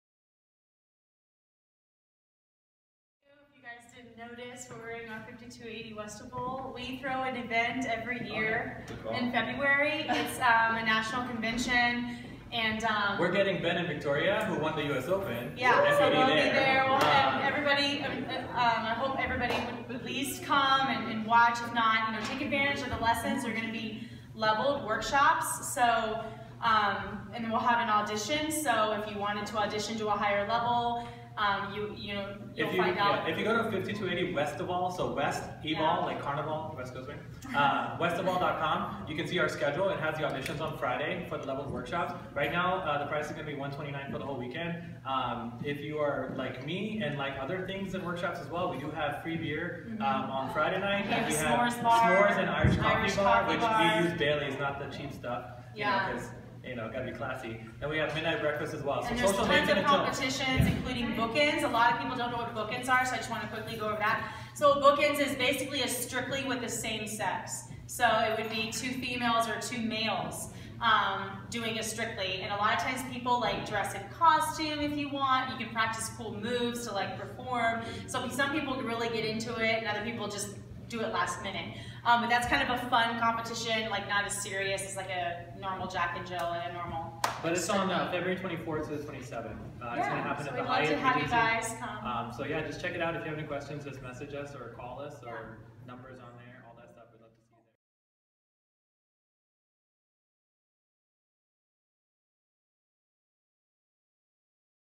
Haider & Aly – 5280 Westival Announcements
Announcements by Haider Khan & Aly Yorek about upcoming 5280 Westival Dance Convention in Denver February 2017, at conclusion of a 3-hour intermediate west coast swing workshop Dec 10, 2016.